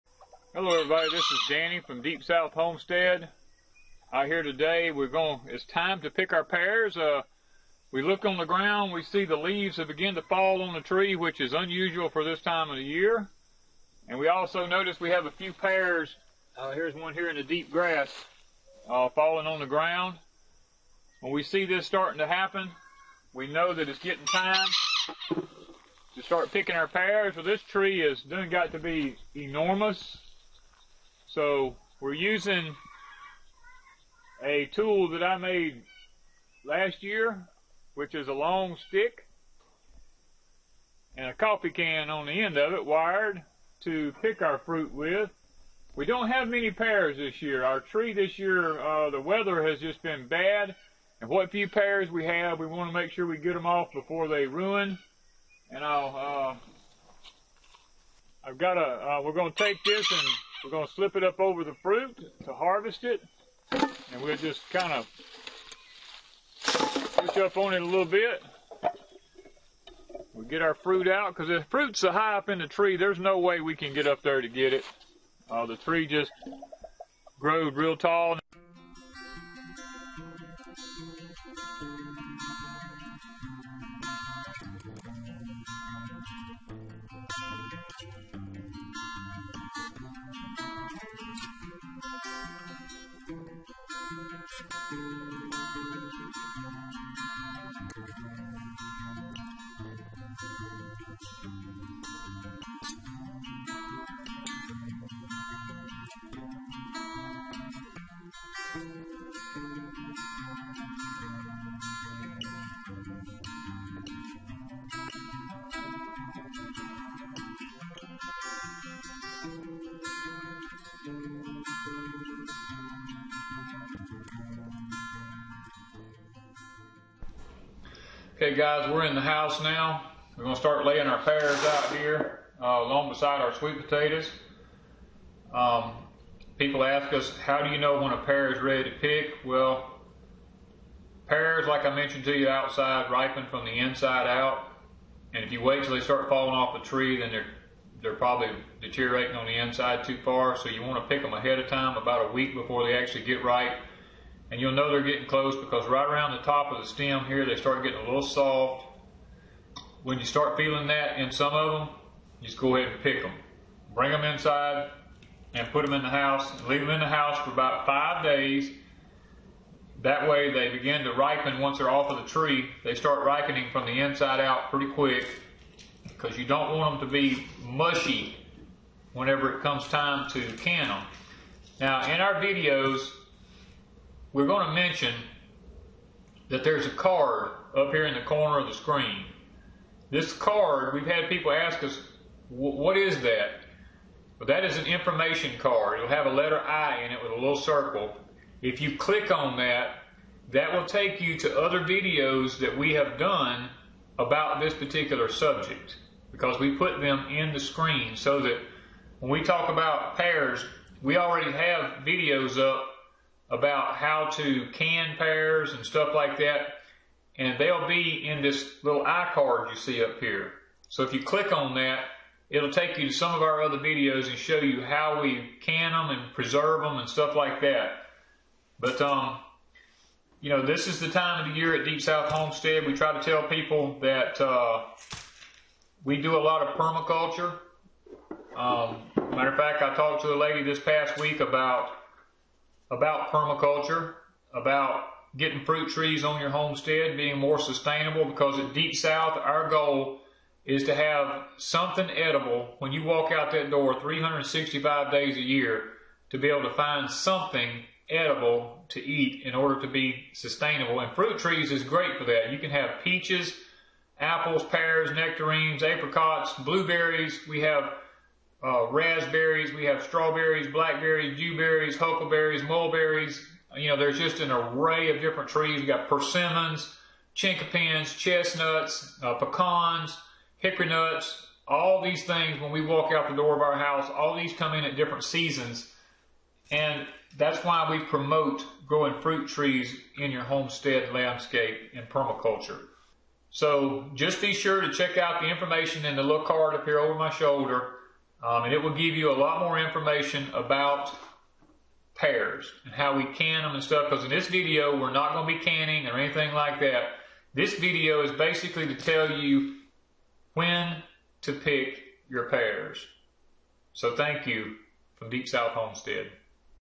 How To Know When PEARS are Ripe
How to know when do you pick pears?  Danny shows you his homemade pear picking tool and tells when to know if your pears are getting ready to be picked.
To Order a hat similar to Danny's

Cooking Southern Style with Deep South Homestead cookbook Sweet Potato Manuel to order